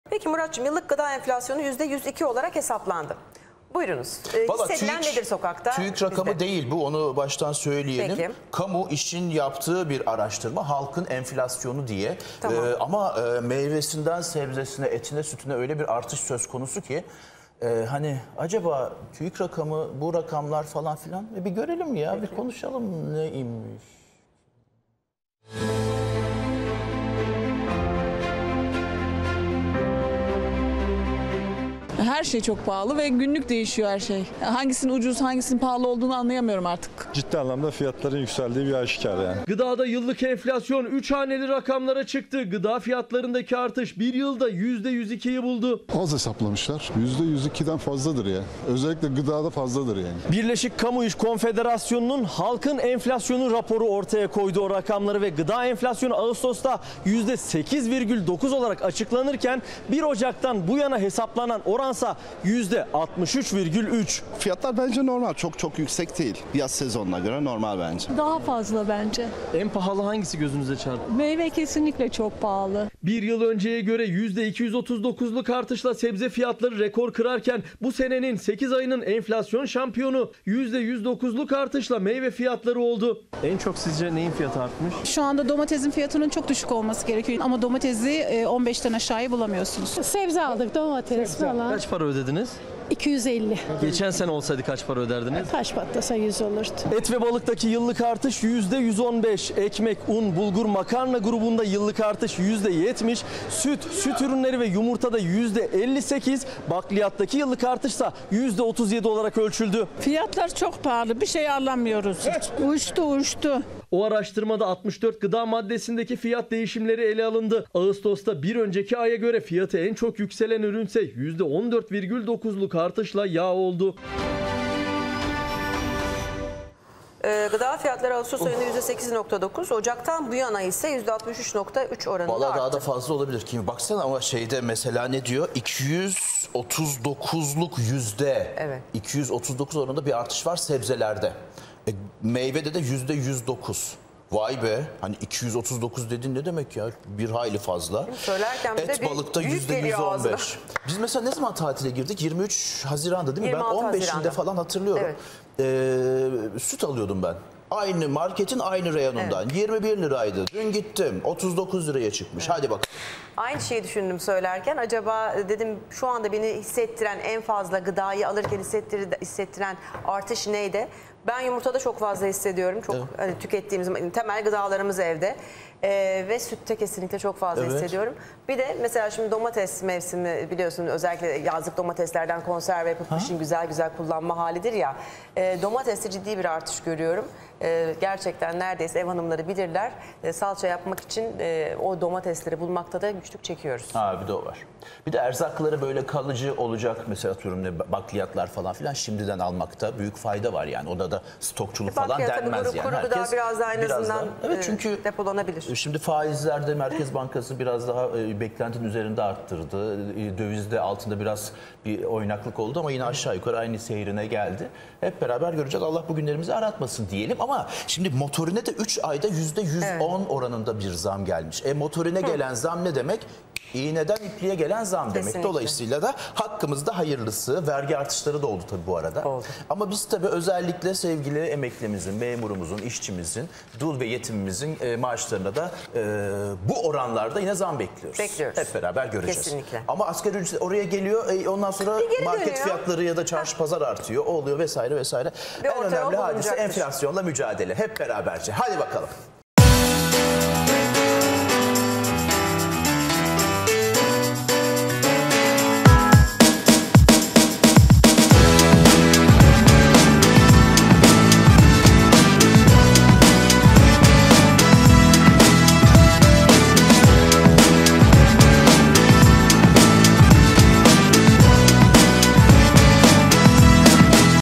Gıda enflasyonu %102 çıktı!
Gıda da yıllık enflasyon 3 haneli rakamlara çıktı. Gıda fiyatlarındaki artış 1 yılda %102'yi buldu.